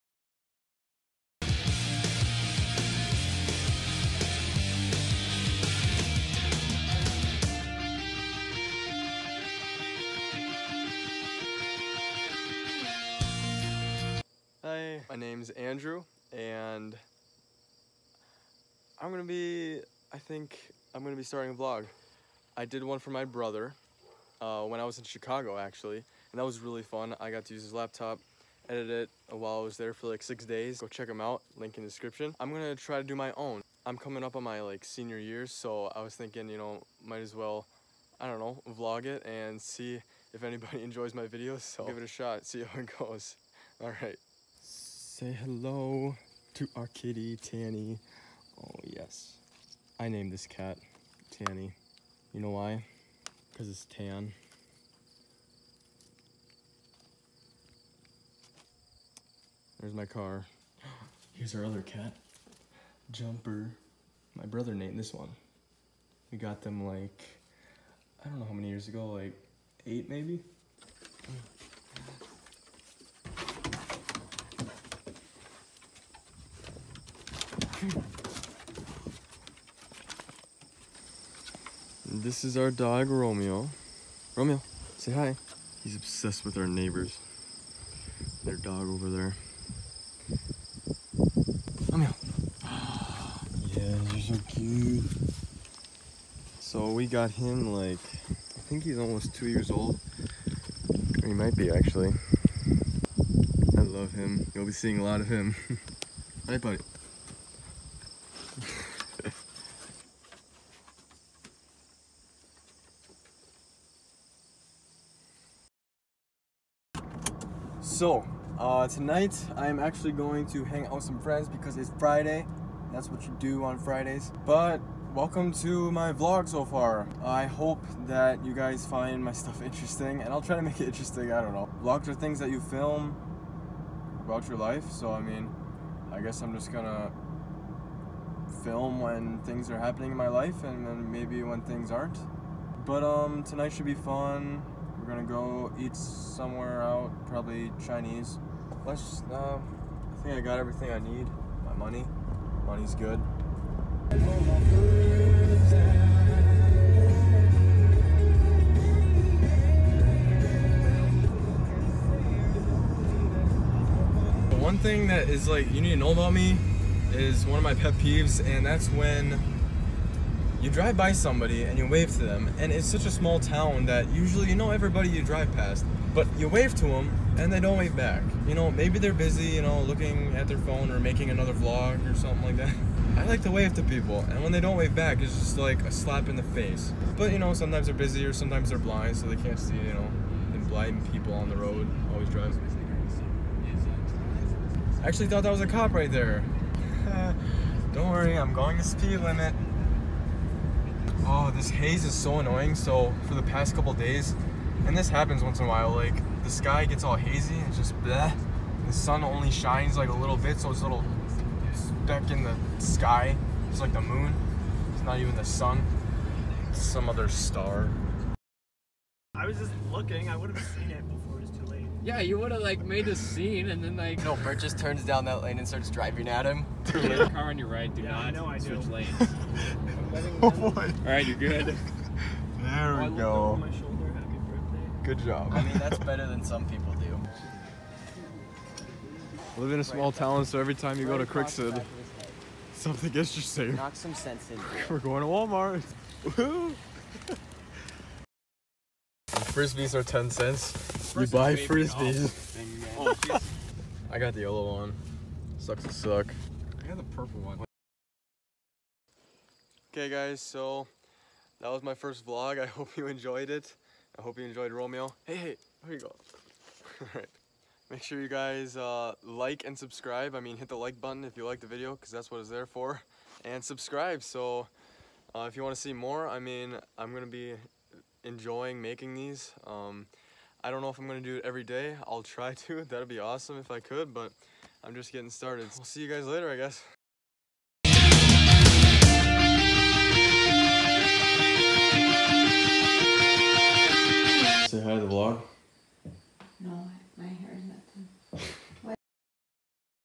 Senior Year Vlog
Hey there! So as you see this is my first vlog of many. I am coming up on my senior year and just wanted to try something new! I have the hardware and software that I need to do this so I gonna give er a shot lol.

Snap-@paperairplane22

From Channel: Lillylover100
Artist: Relient K